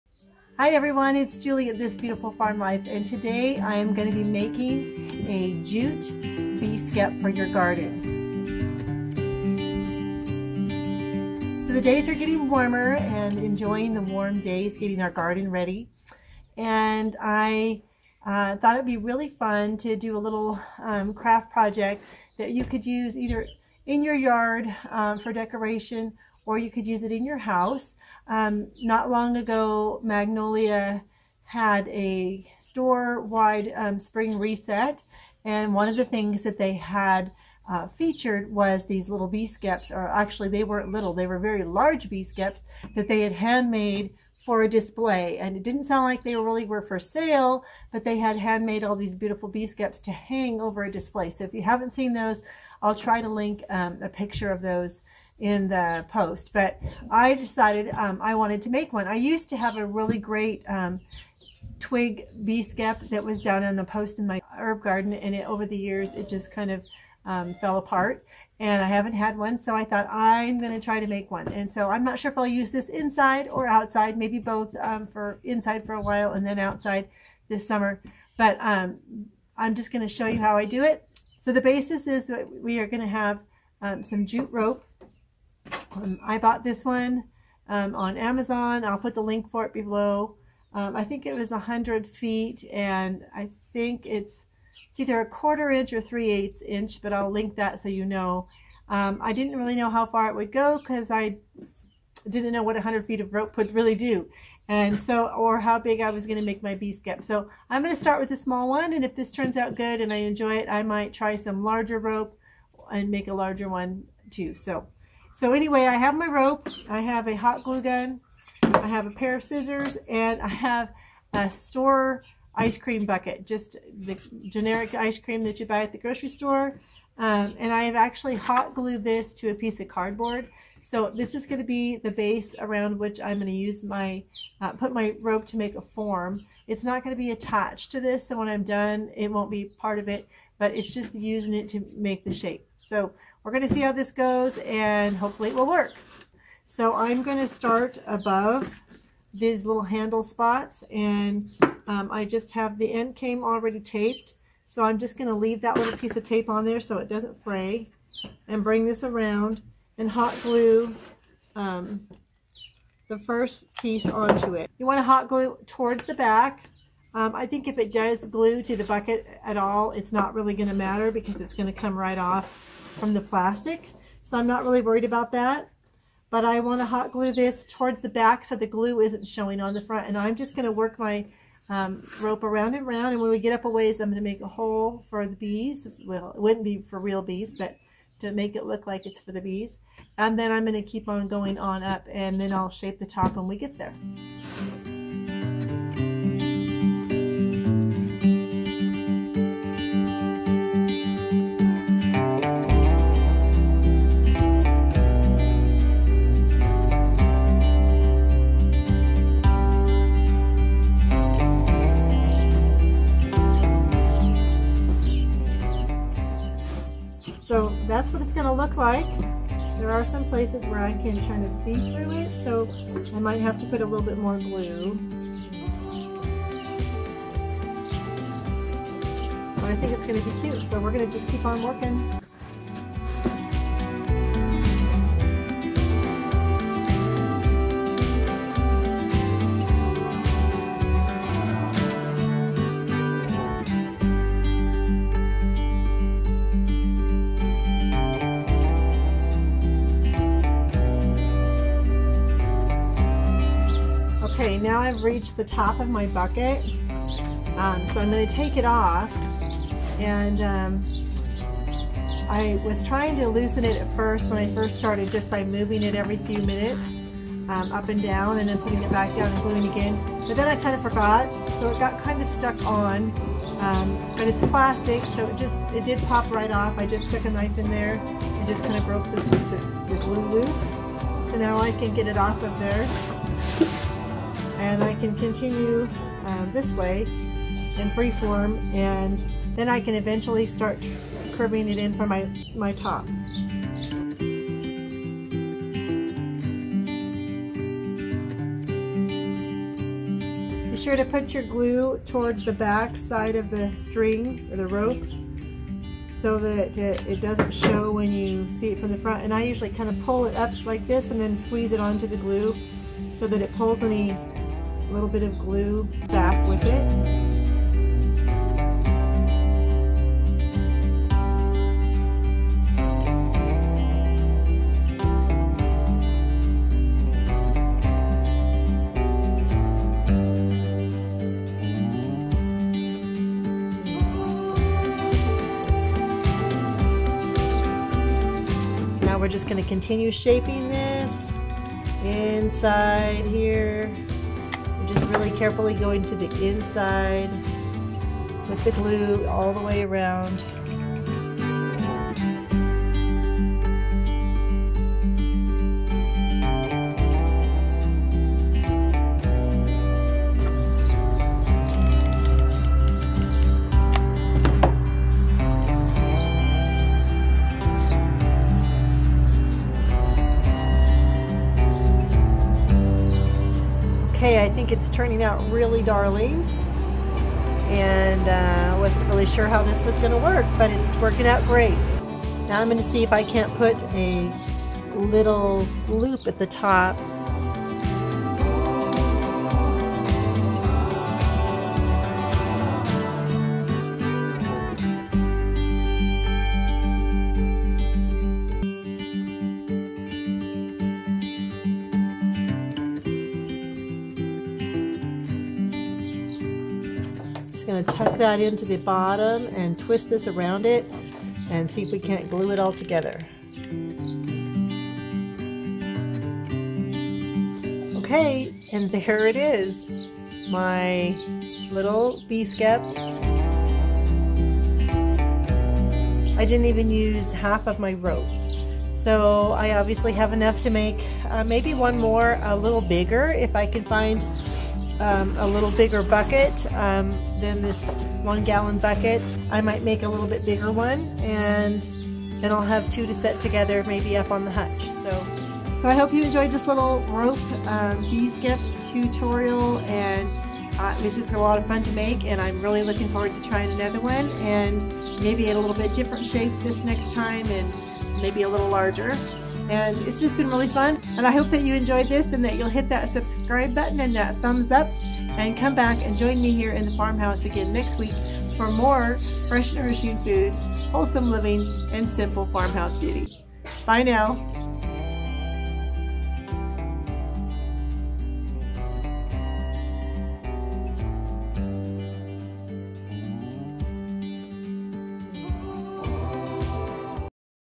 How to Make a Decorative Bee Skep
LEARN HOW TO MAKE A DECORATIVE BEE SKEP TO DRESS UP YOUR HOUSE OR PORCH THIS SPRING.   JUST A FEW SIMPLE SUPPLIES & 30 MINUTES IS ALL YOU NEED!

Thanks for stopping by This Beautiful Farm Life!